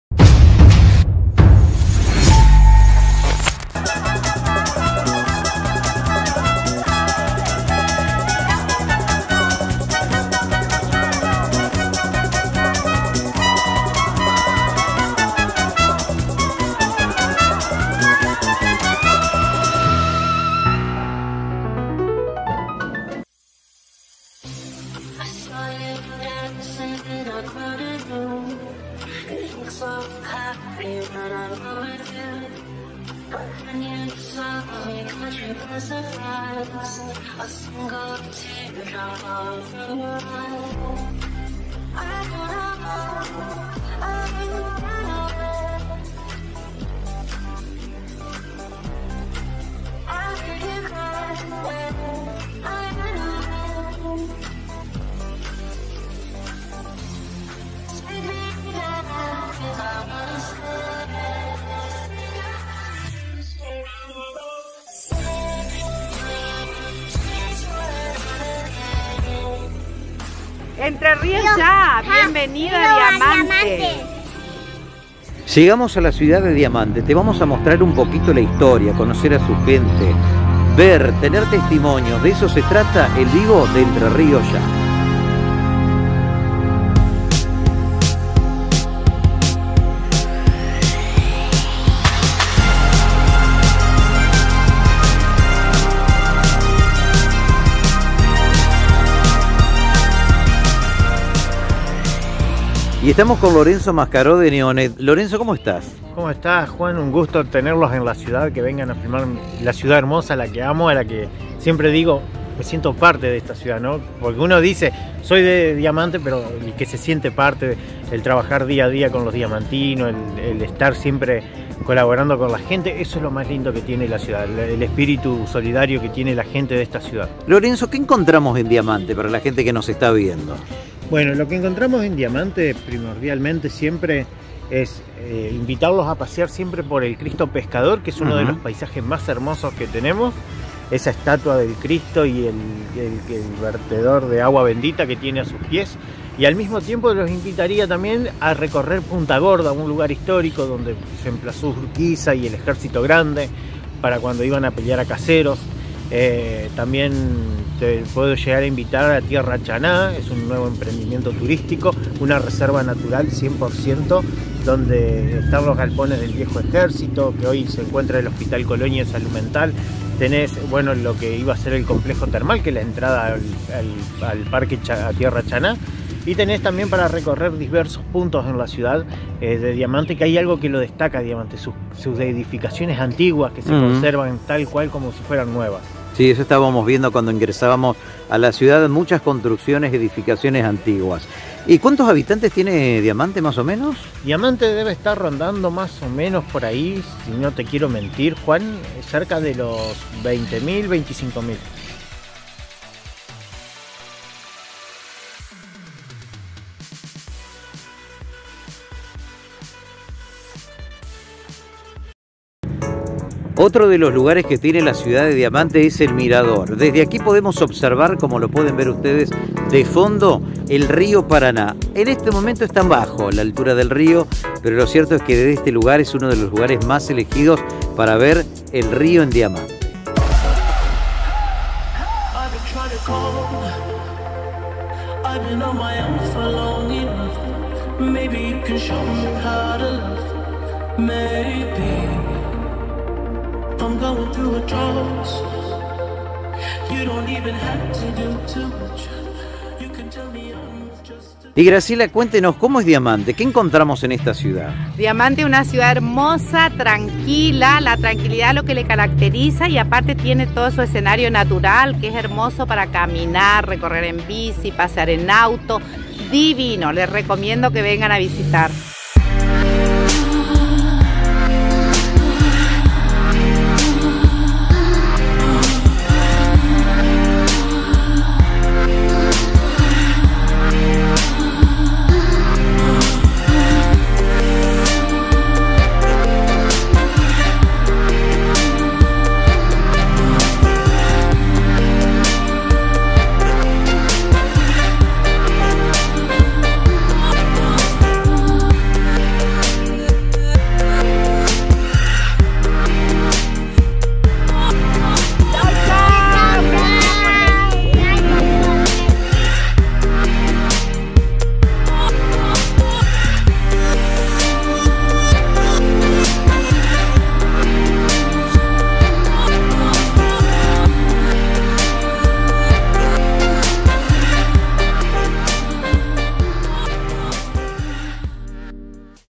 Diamante, Entre Ríos.
La cámara de EntreRíosYA visitó la ciudad de Diamante en la provincia de Entre Ríos.